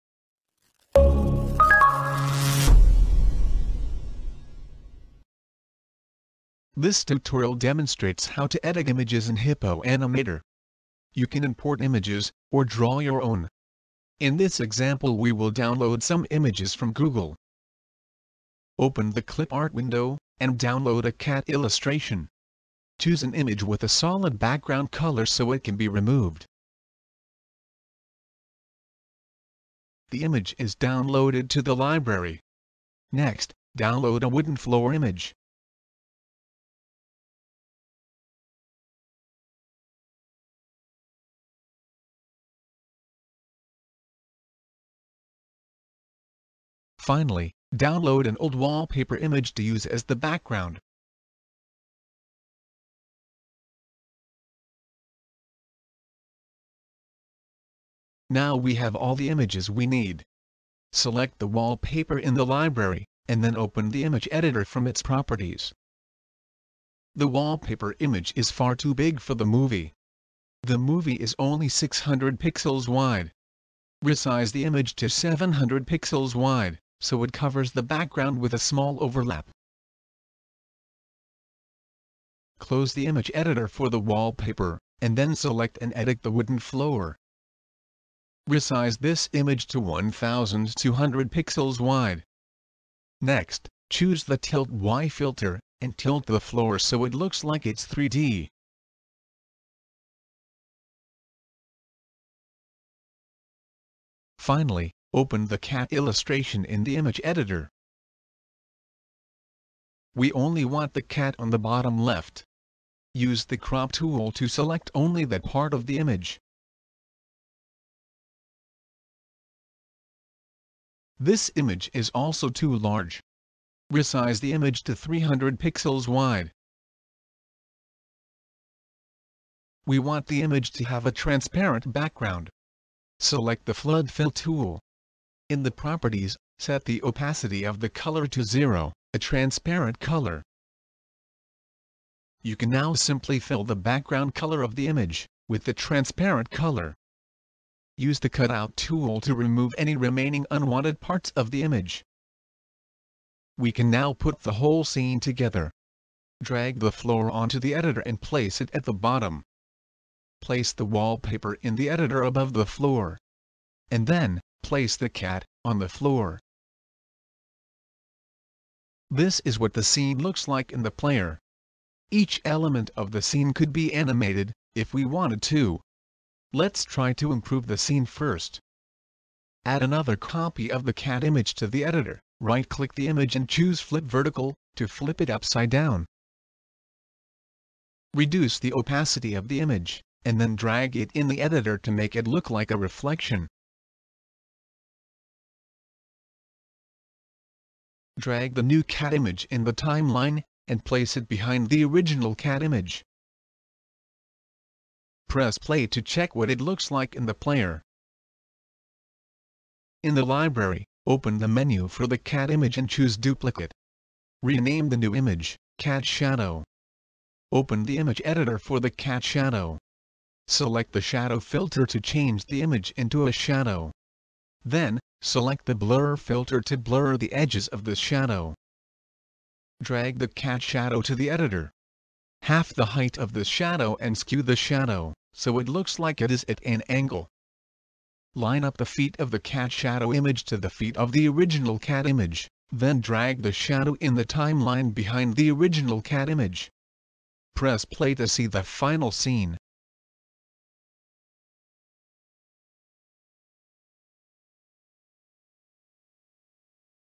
Using the Image Editor - Hippo Animator 3 Tutorial
This tutorial demonstrates several features in the image editor and uses them to build a scene.